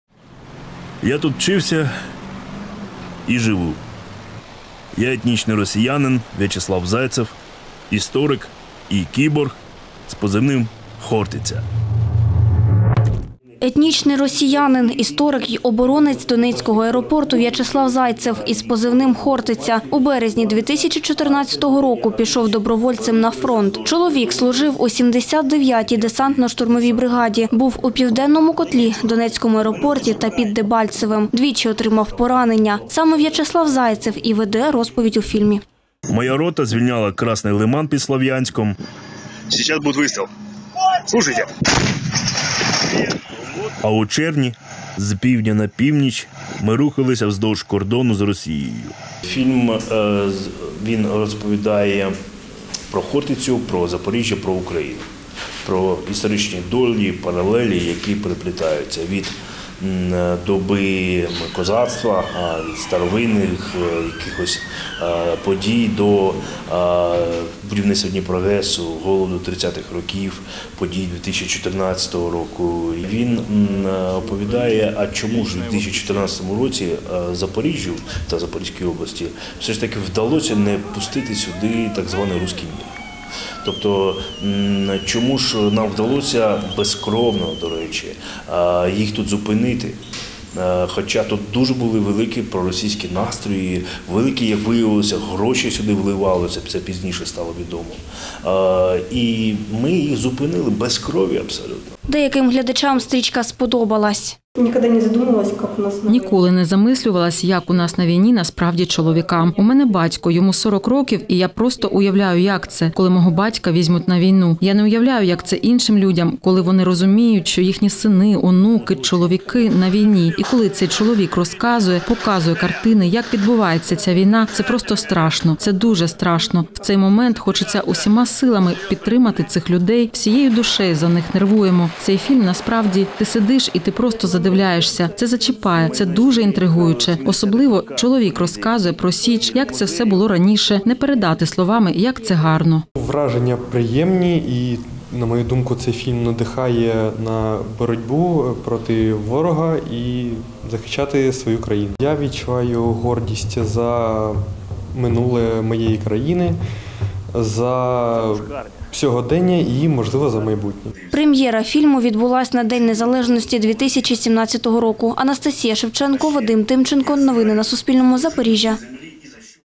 Новини - Презентація фільму "Січ" - 07.03.2020
У Запоріжжі відбувся показ документального фільму «Січ». За словами організаторів перегляду в Запорізькій обласній бібліотеці для юнацтва, стрічка виробництва «Радіо Свобода» є спробою дослідити, що формує «імунітет» до вірусу «русского міра».